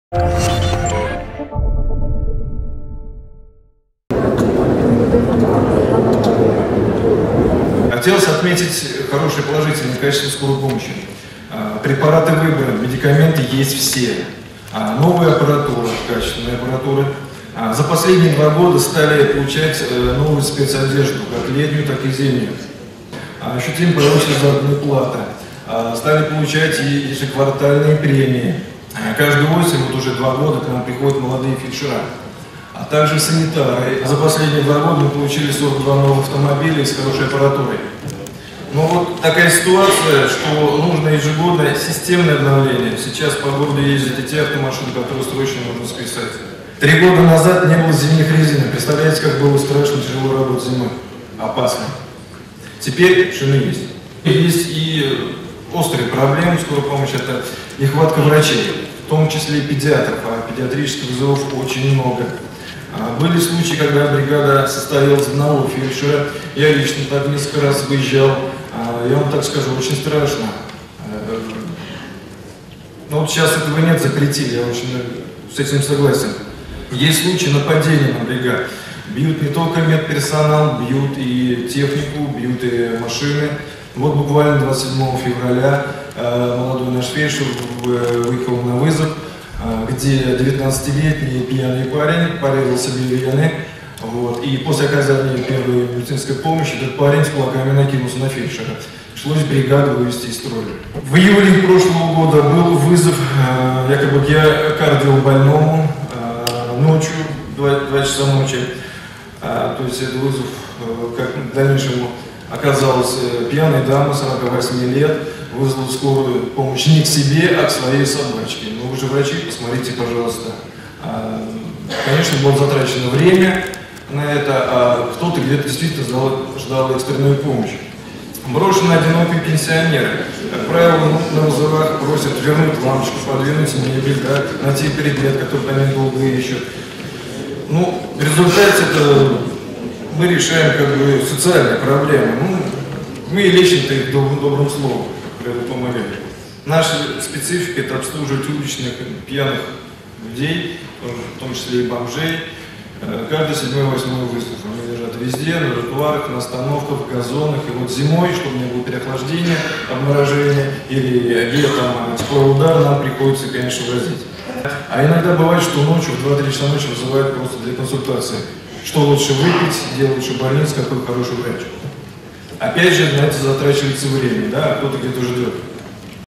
«Скорая»: каждый 8-й вызов к бомжам
За 2014 год медики «скорой помощи» Самарской области выезжали на 687,2 тысячи вызовов. Эти данные озвучили представители территориального фонда обязательного медицинского страхования региона (ТФОМС).